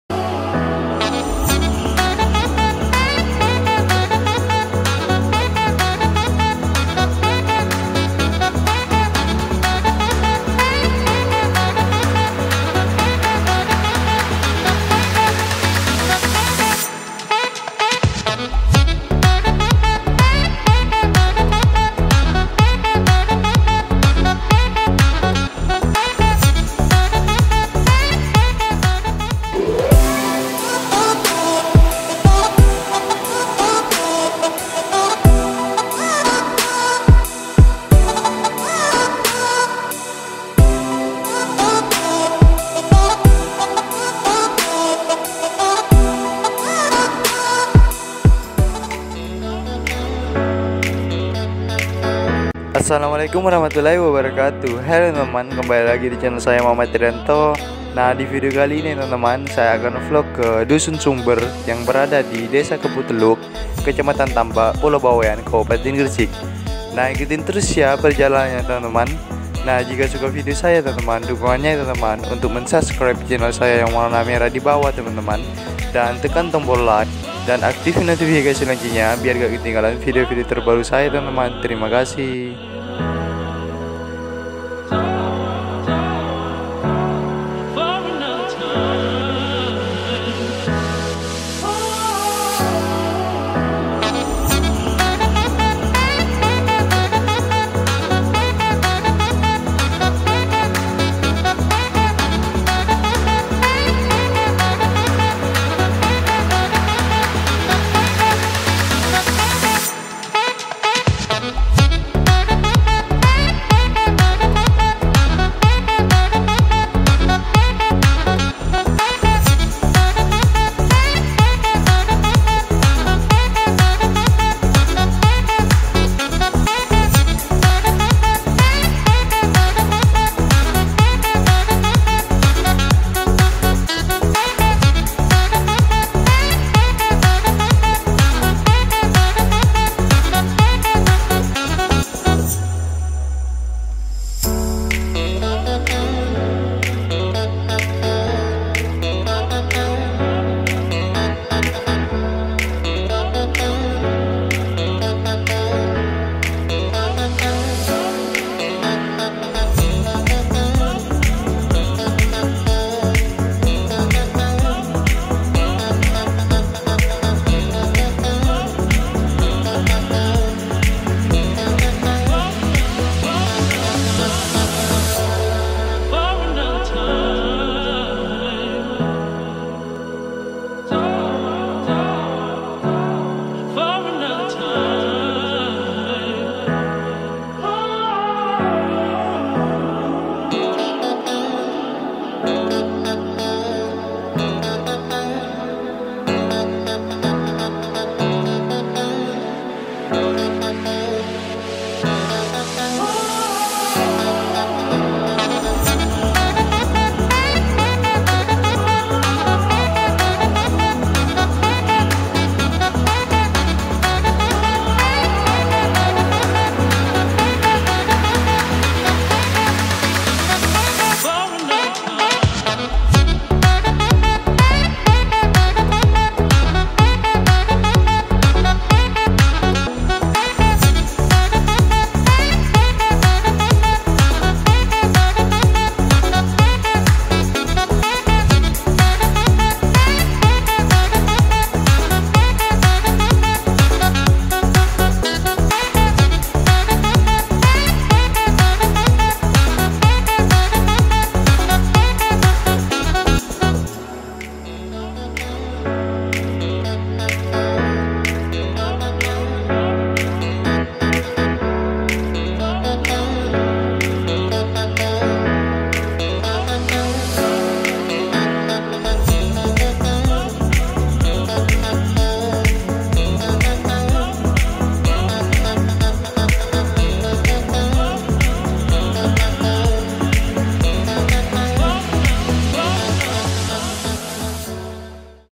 VLOG KEINDAHAN DUSUN SUMBER DI PULAU BAWEAN
Yang penasaran dengan keindahan Dusun Sumber tonton videonya sampai habis ya,karna di dalam video ini pemandangan yang ada di dusun Sumber sangat indah dan sangat alami.

Dusun Sumber ini berada di Desa Kepuhteluk,Kecamatan Tambak,Pulau bawean,Kabupaten gresik,Jawa timur.